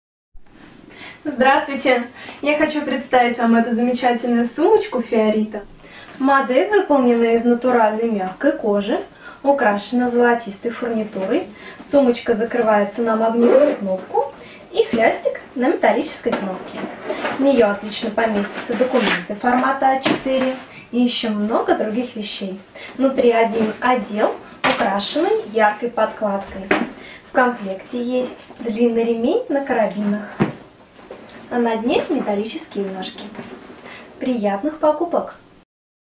Сумка Fiorita 240 cream blue
Великолепная сумочка из высококачественной натуральной кожи от Fiorita. Модель дополнит образ и станет любимым аксессуаром на каждый день. Сумочка украшена золотистой фурнитурой и контрастными вставками. Закрывается на магнитную кнопку, носится на сгибе руки. На дне имеются металлические ножки. Яркая внутренняя отделка.
Внутри: на задней стенке карман для документов на молнии, на передней — два кармана для мелочей и маленький кармашек на молнии.